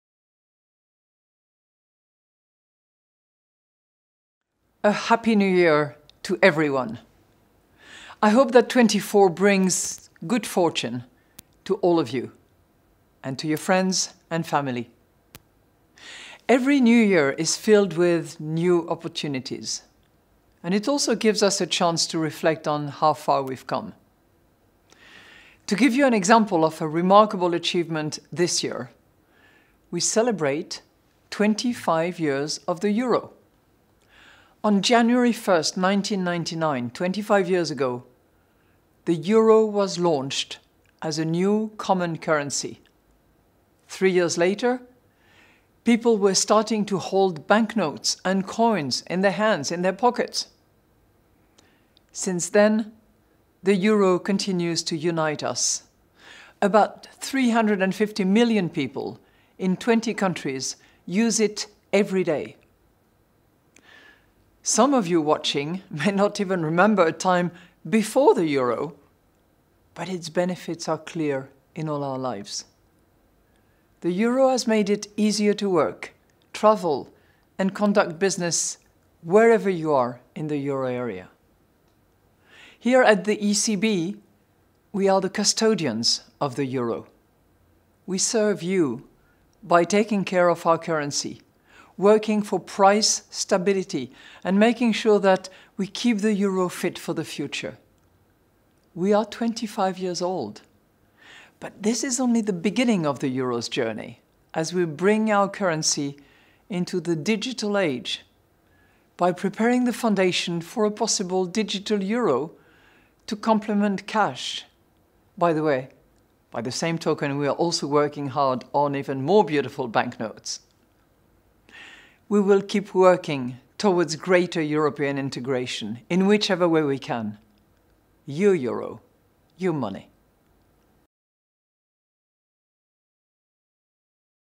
New Year’s message from President Christine Lagarde
European Central Bank President Christine Lagarde wishes viewers a healthy and successful 2024. She references the 25th anniversary of the euro as a cause for celebration in the new year.